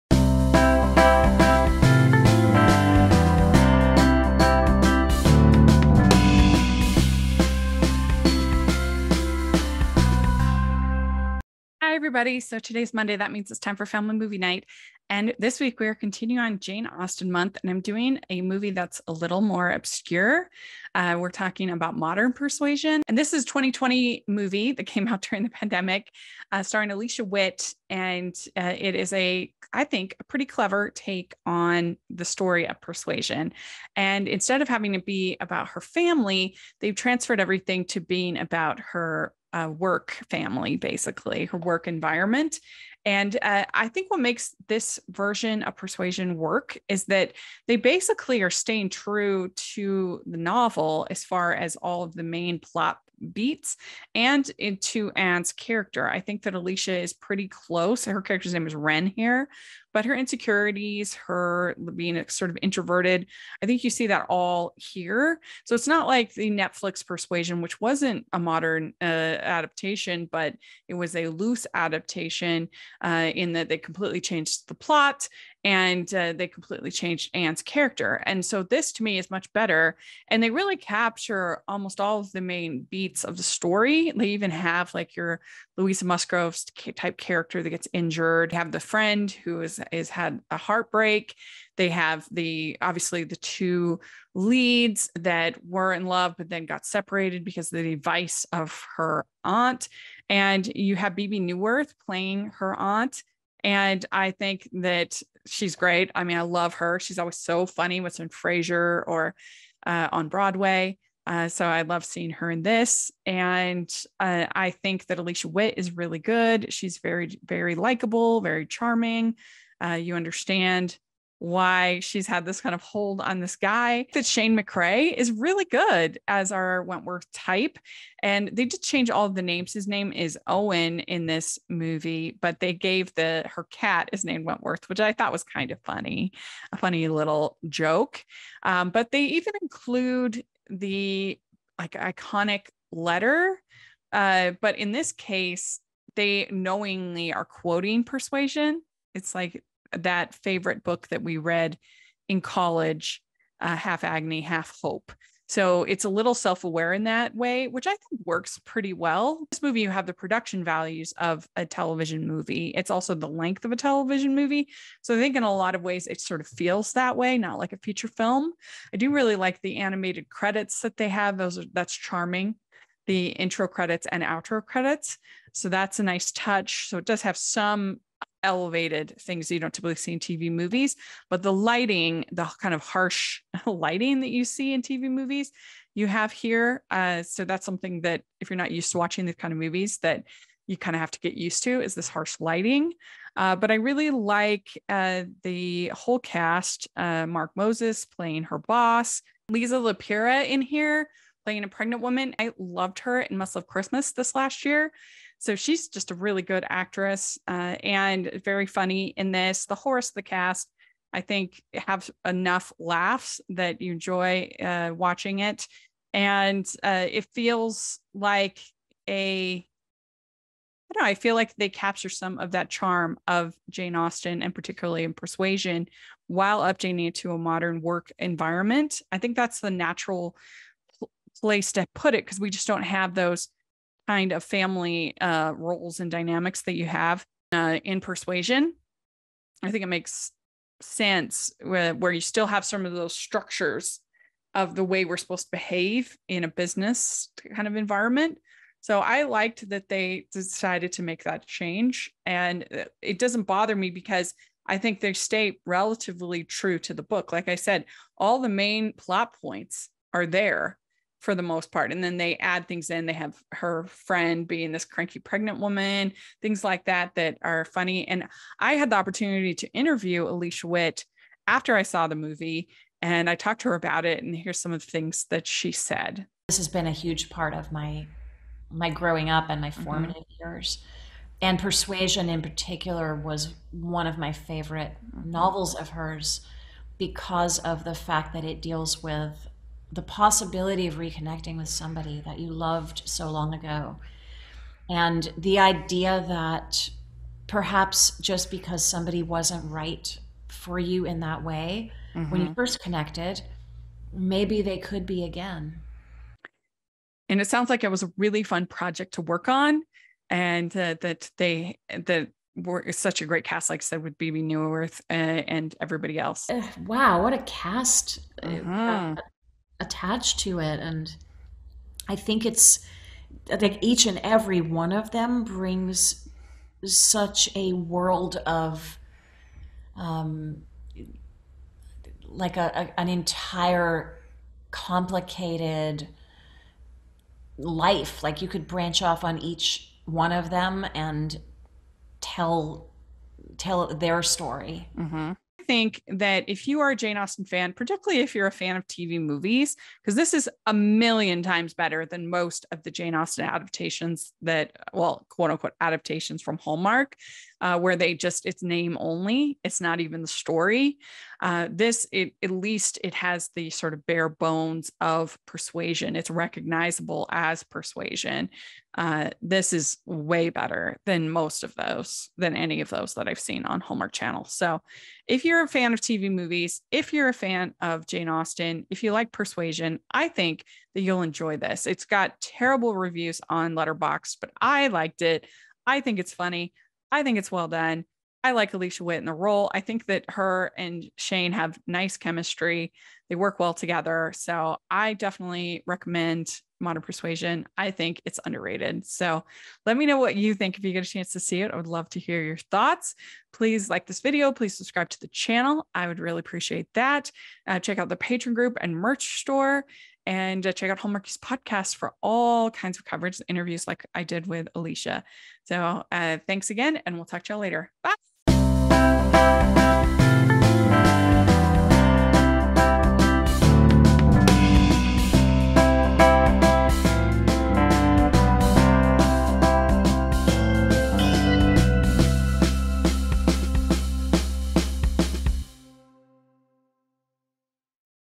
A Defense of MODERN PERSUASION (Jane Austen Month) (Alicia Witt Interview)
Today we are looking at a hidden gem from 2020 the modern adaptation of Jane Austen's Persuasion entitled MODERN PERSUASION